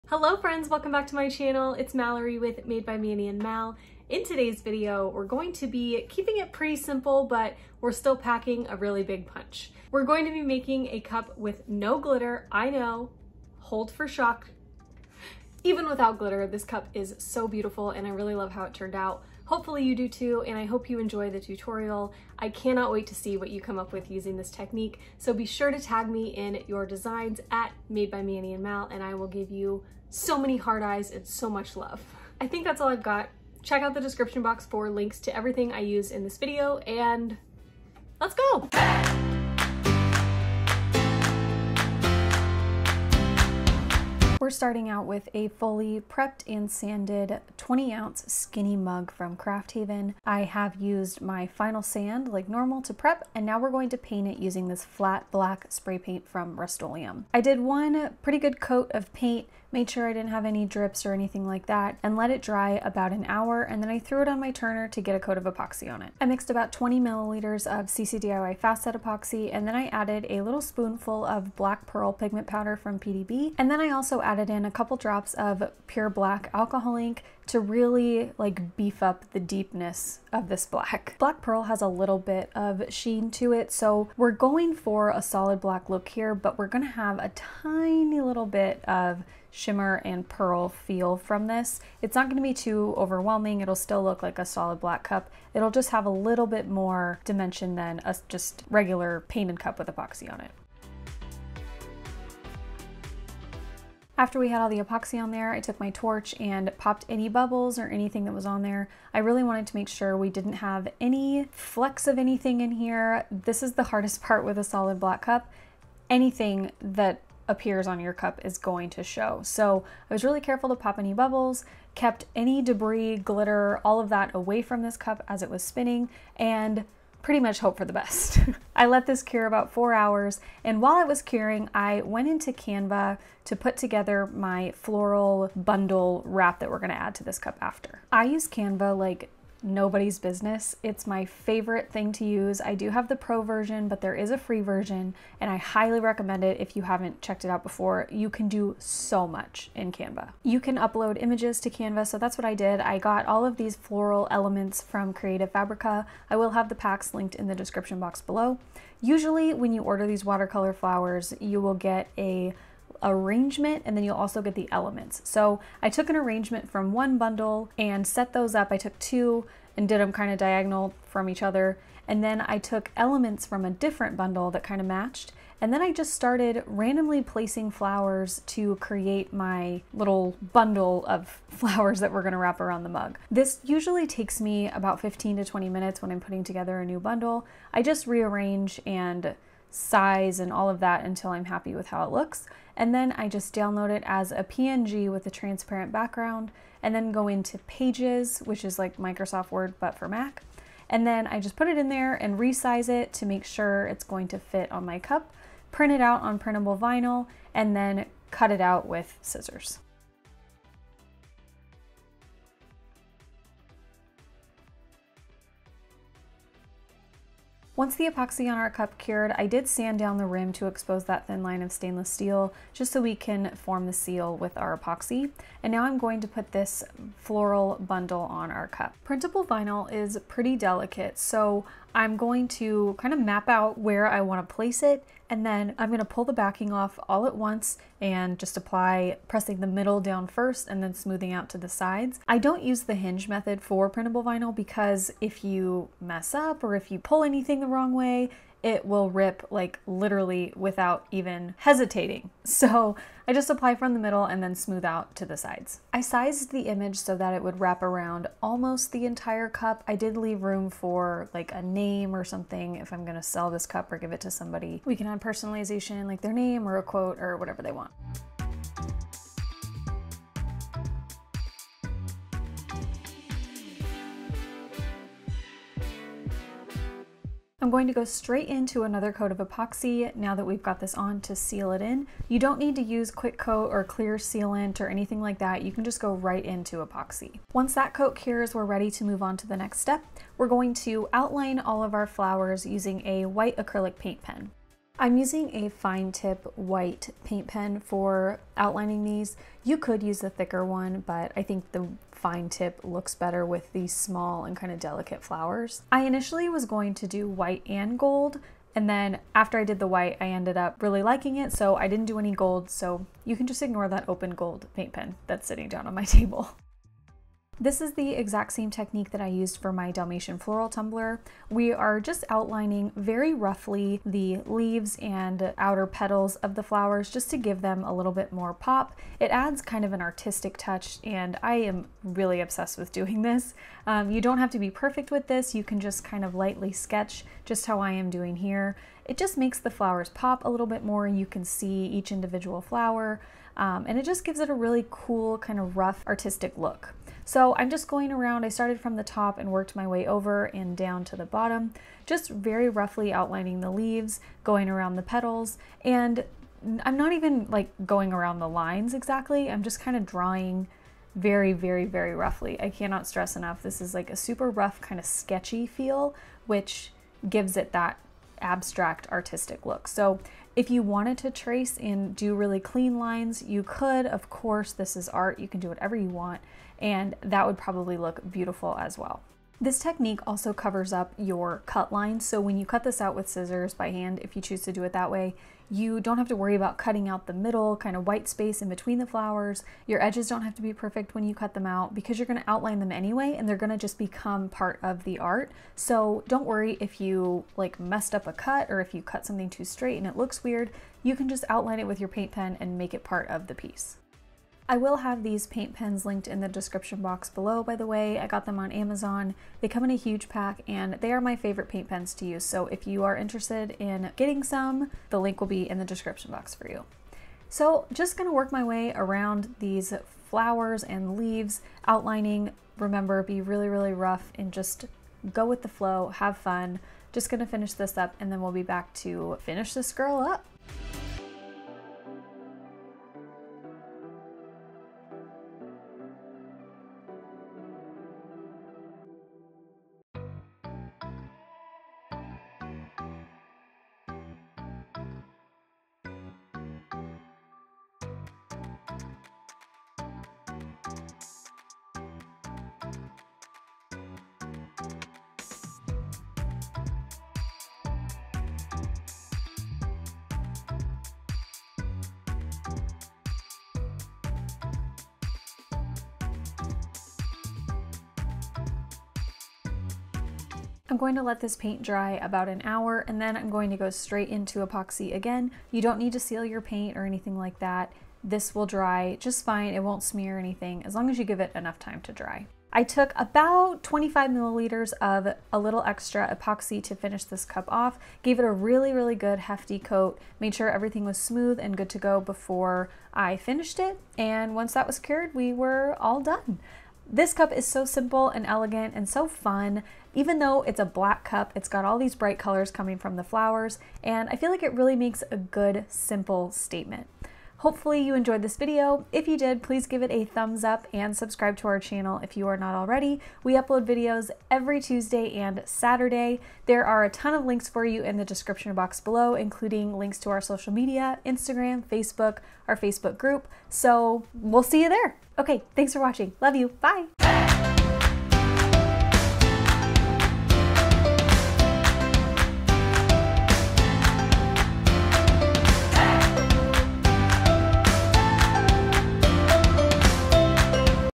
Solid Black Mug With Bright Flowers - Non-Glitter Tumbler Tutorial
Hello Friends! Welcome back!

In this video, we’re keeping it simple and elegant with a solid black & bright floral tumbler. I hope you enjoy it!

💠 Shop our Products:
Made By Mani & Mal
Made By Mani & Mal Digital Collective

💠 SUPPLIES USED IN THIS VIDEO:
- TUMBLERS: 20 oz. Skinny Mug 👉🏼 Craft Haven crafthaven.com
SPRAY PAINT: Flat Black 👉🏼 Rustoleum
- PIGMENT POWDER: Black Pearl 👉🏼 PDB Creative Studio 🎉 Code MALLORY21 gets you a sweet lil discount!
- ALCOHOL INK: Black 👉🏼 Brea Reese (3 pack) /
- EPOXY: Counter Culture DIY Fast Set 👉🏼 // A Little Extra, Inc 👉🏼 aleshine.com
- PRINTABLE VINYL
- PAINT PENS
- ELECTRICAL TAPE
- METAL STIR STICKS
- SANDING BLOCKS
- NITRILE GLOVES
- VINYL SCRAPER